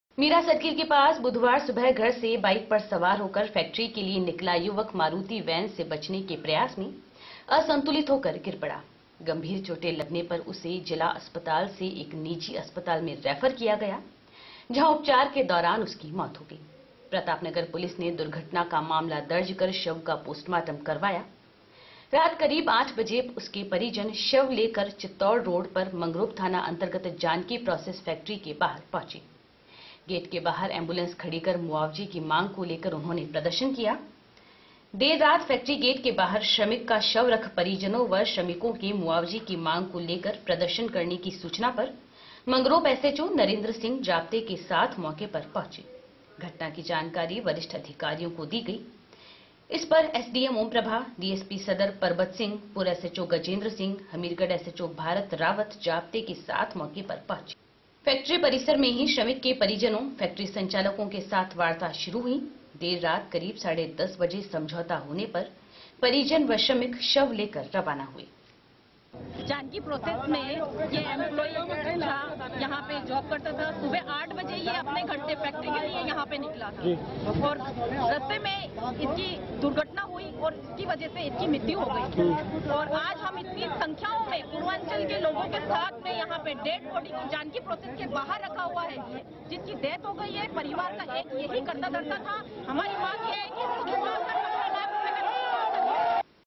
Bhilwara Lokwani News 23082018
मुआवजे की मांग को लेकर फैक्ट्री गेट पर शव राखकर किया प्रदर्शन. यदि आप के पास है कोई न्यूज़ ( ख़बर ) / न्यूज़ का वीडियो / न्यूज़ के फोटो तो हमें इस ईमेल एड्रेस पर भेजे